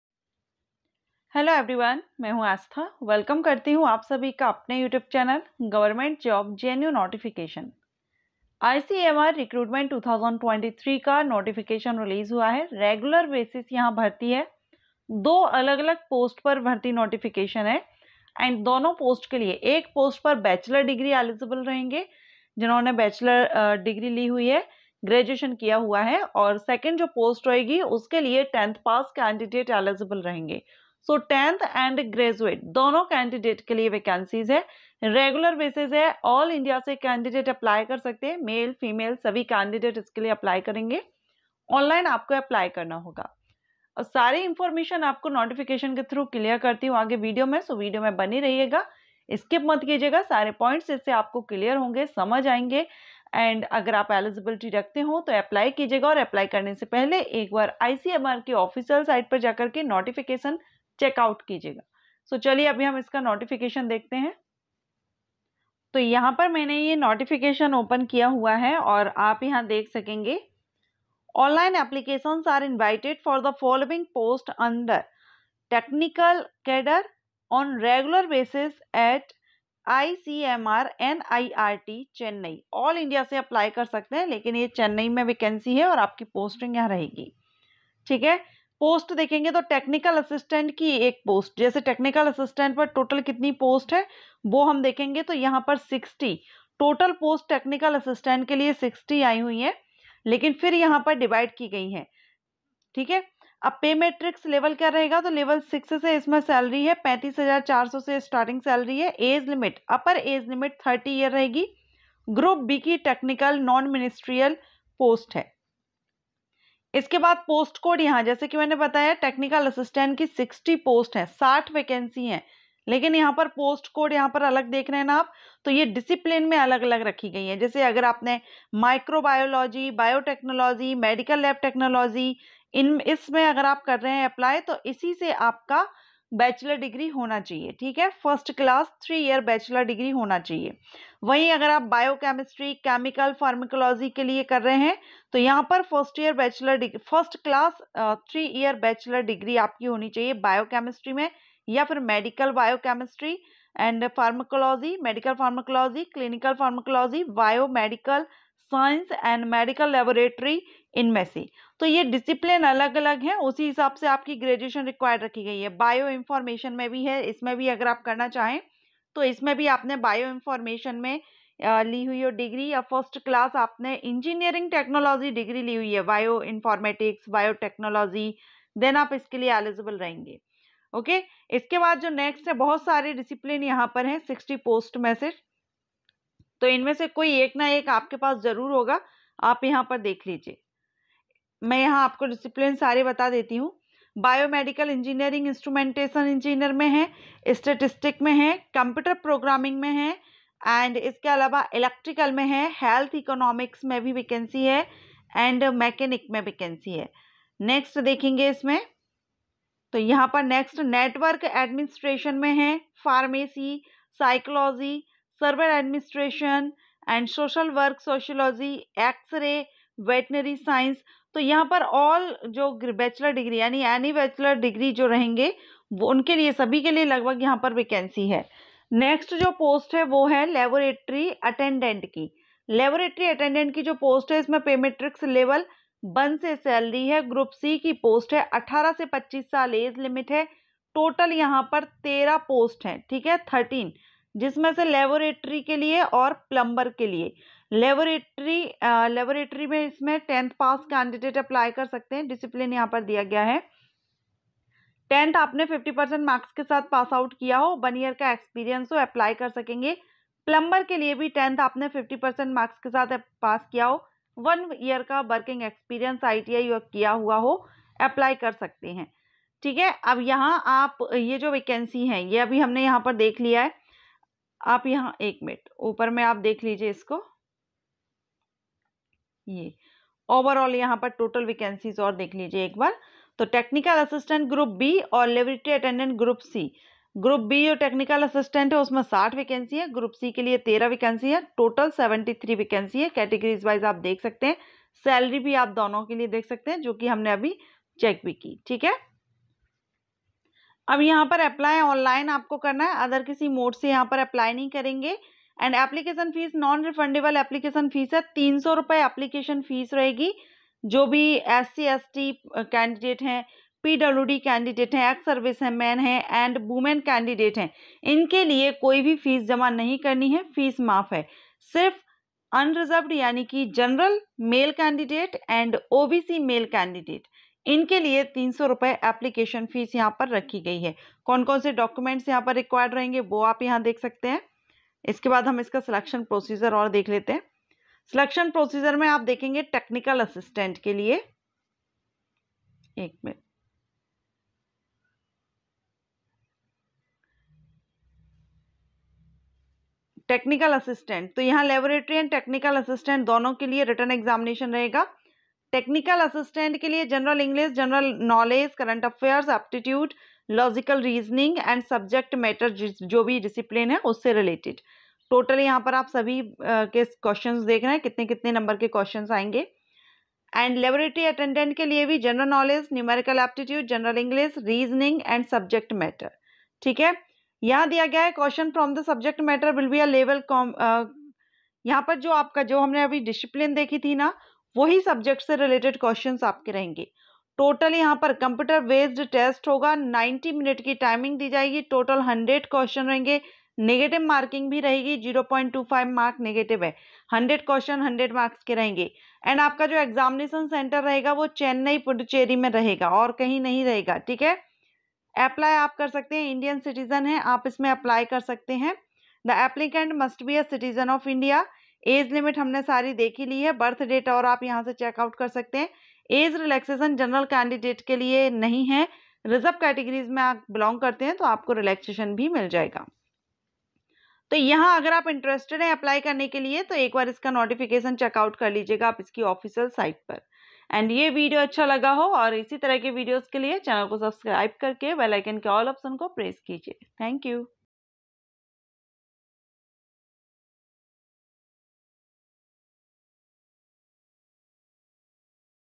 ICMR Recruitment 2023 regular basis bharti for 10th AND graduation level | new vacancy notification
Friends New vacancy notification out hua hai regular basis if you you interested you can apply